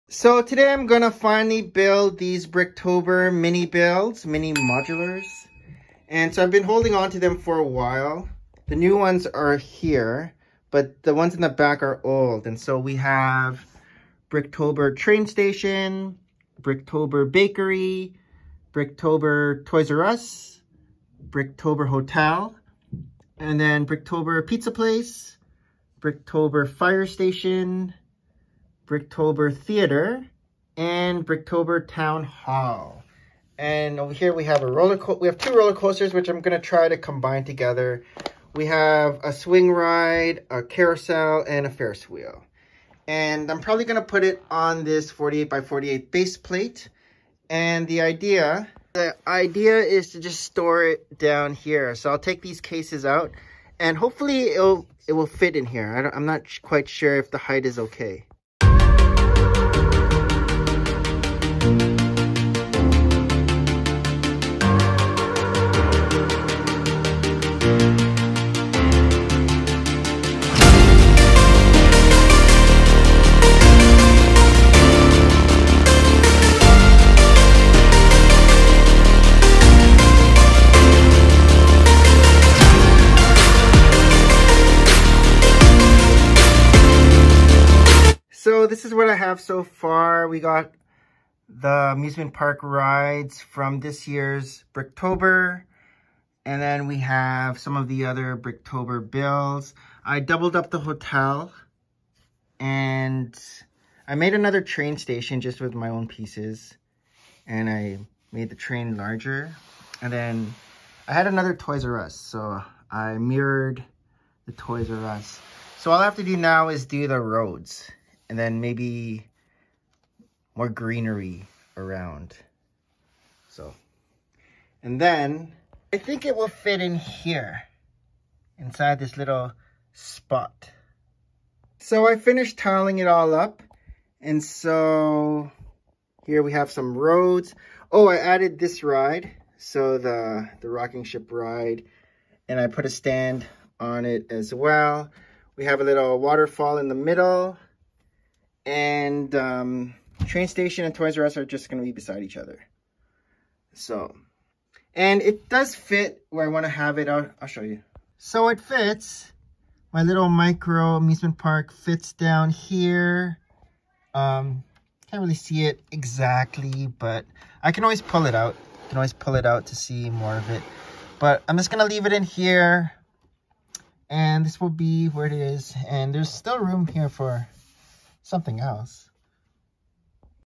My Full Lego Room Update - Week 61- Building Bricktober Sets
This is my full LEGO Room update. I give a complete Lego room update for the 3rd week of April 2022